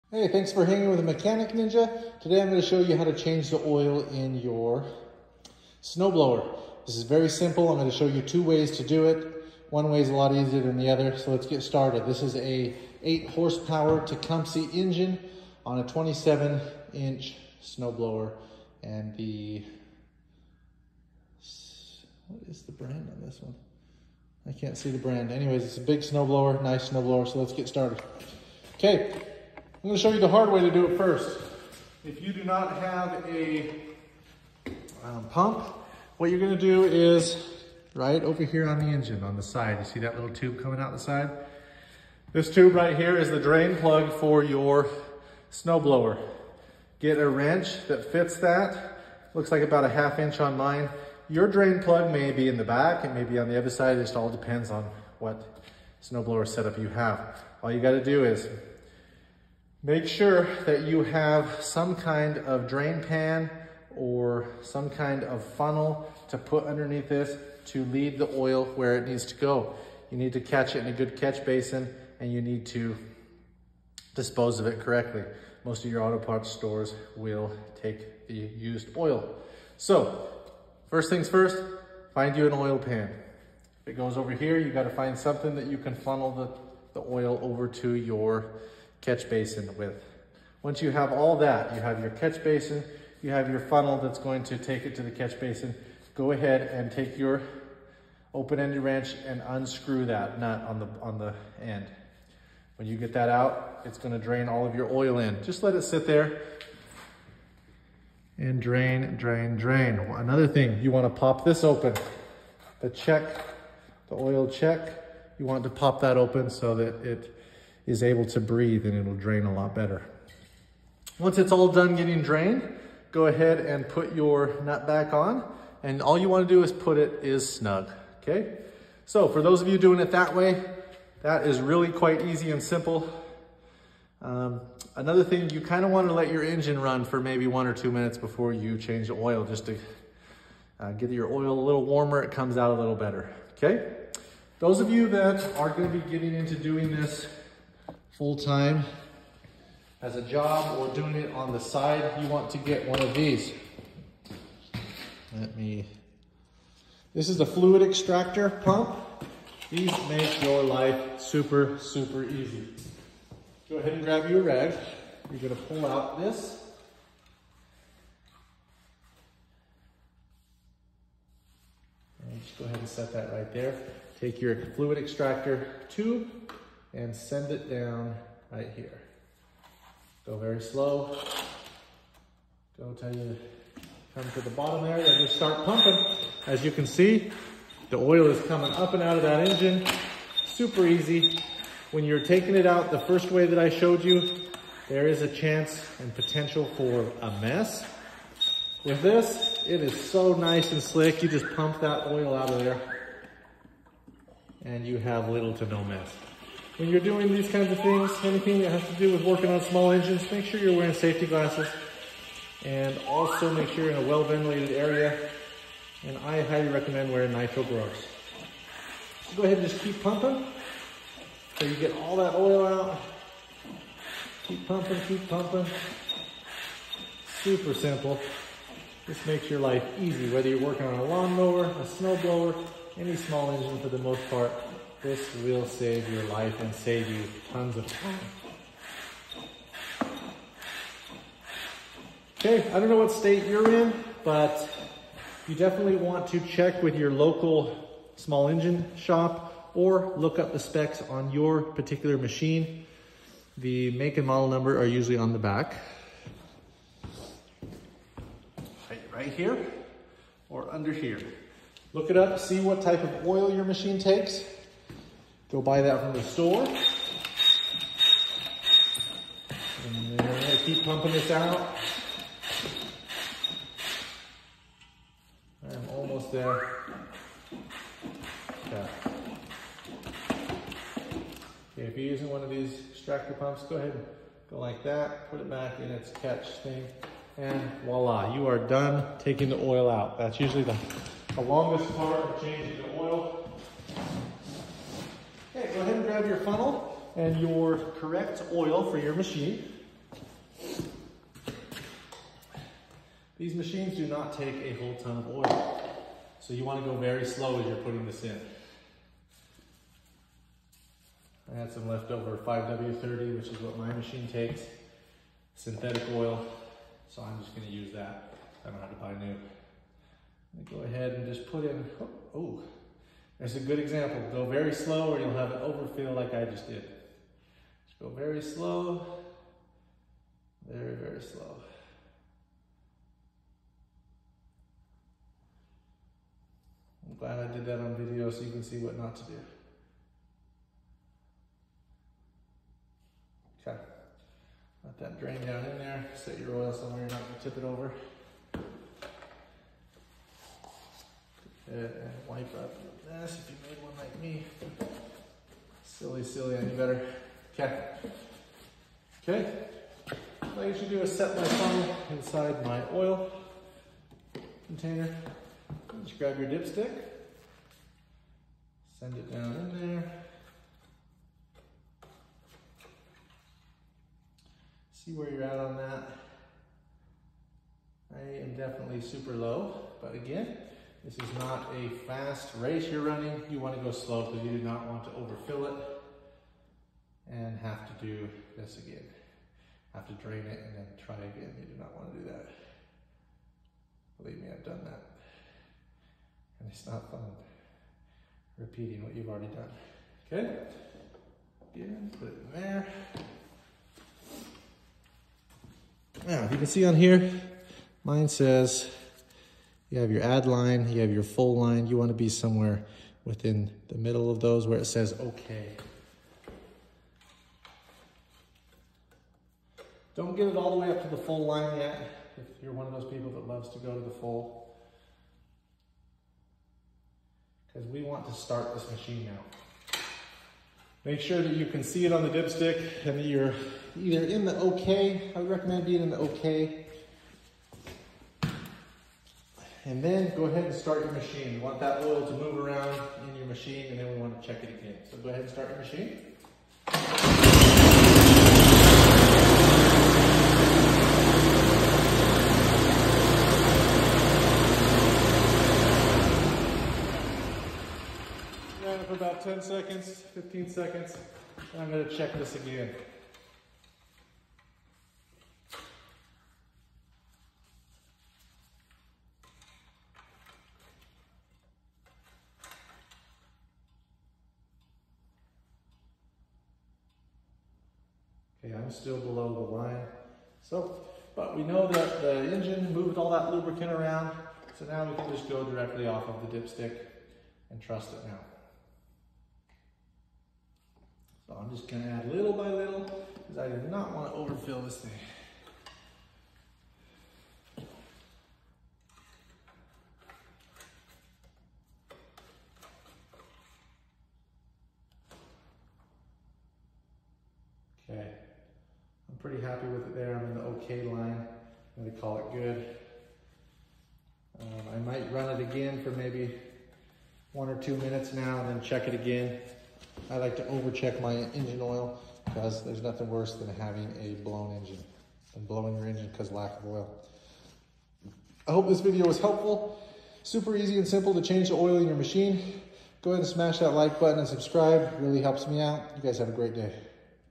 How to easily change the oil in your snowblower! Snowblower repair!
In this video I show you how to easily change the oil in your Tecumseh engine snowblower!